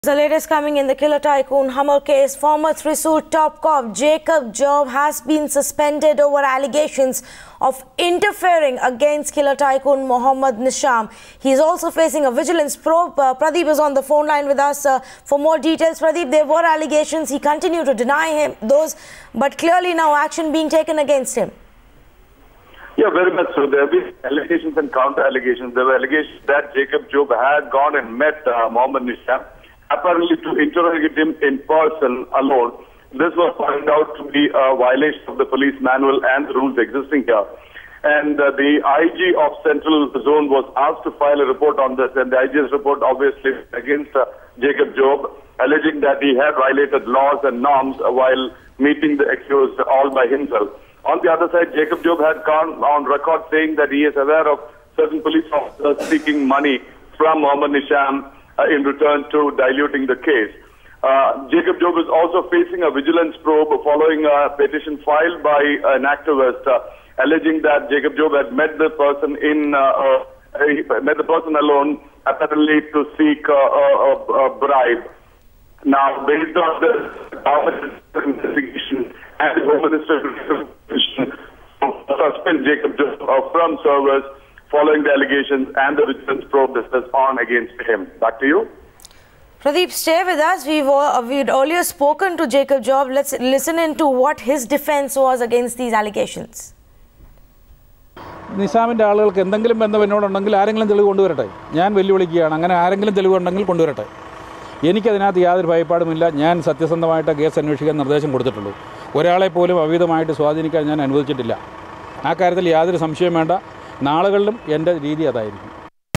Former Thrissur City Police Commissioner Jacob Job suspended
Former Thrissur City Police Commissioner Jacob Job suspended over allegations of interfering in probe against killer tycoon Mohammed Nisham